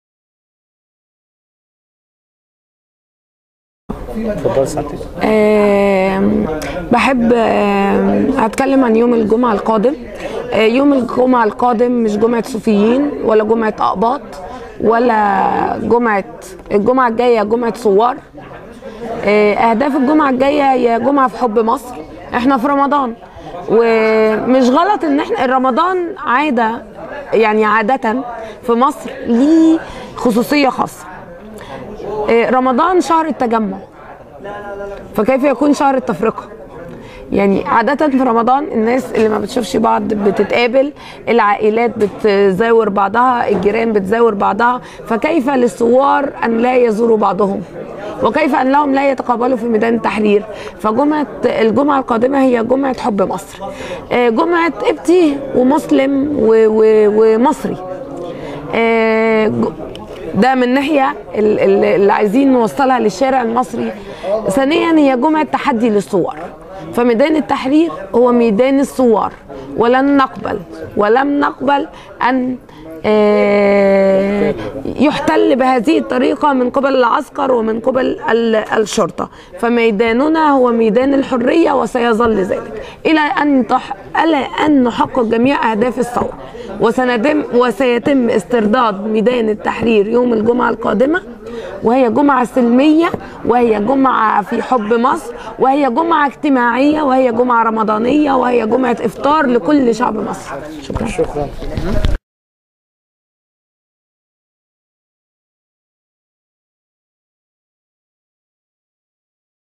أحد شبات الثورة تشرح أهداف المؤتمر
أحد شبات الثورة تشرح أهداف المؤتمر و الأستاذ/ ممدوح حمزة يدعو للجمعة القادمة